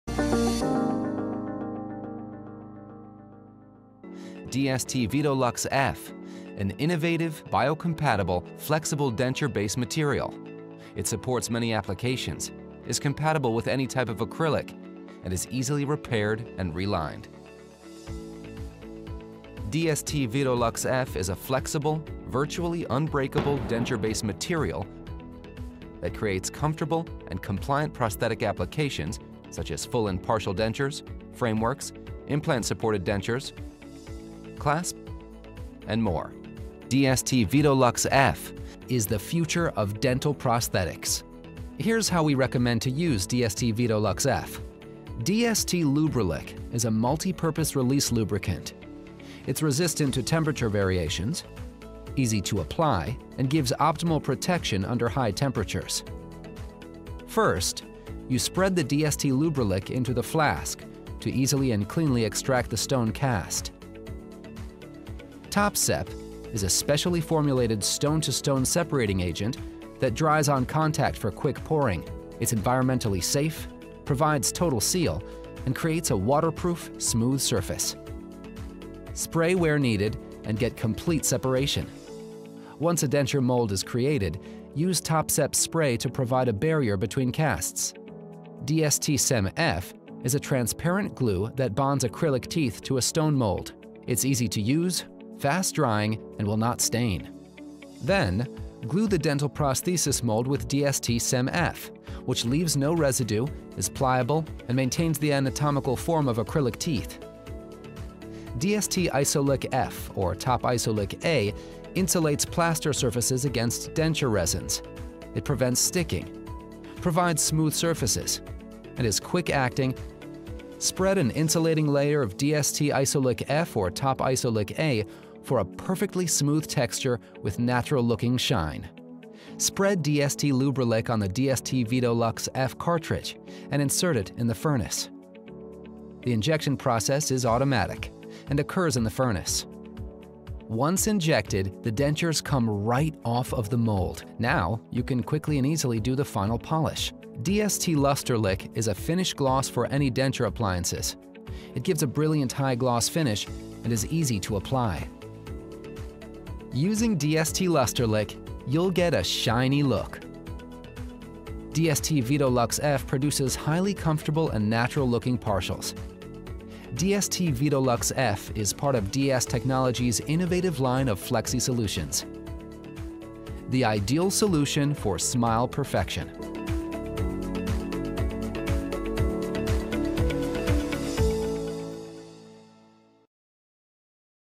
Dental Laboratory Equipment (DS TECHNOLOGY)
We present to your attention materials for dental laboratories from the manufacturer DS TECHNOLOGY